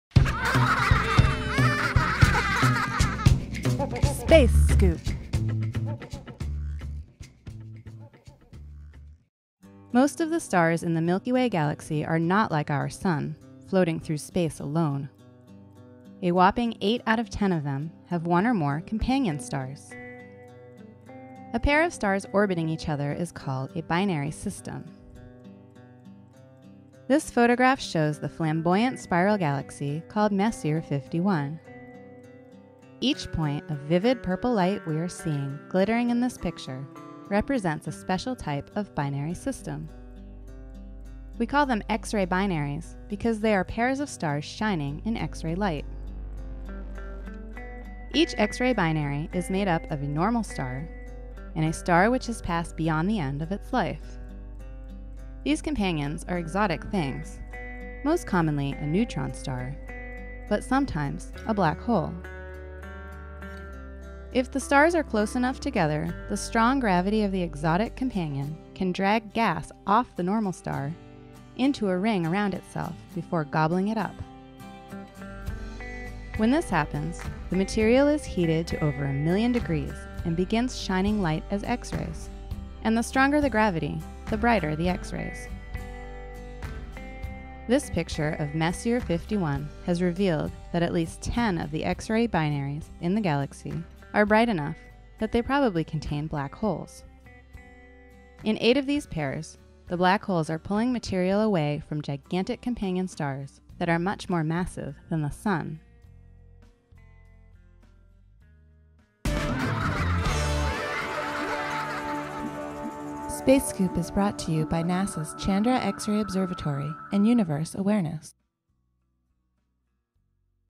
Space Scoop: X-rays Uncover Black Holes Dancing With Normal Stars
This photograph shows the flamboyant spiral galaxy called Messier 51.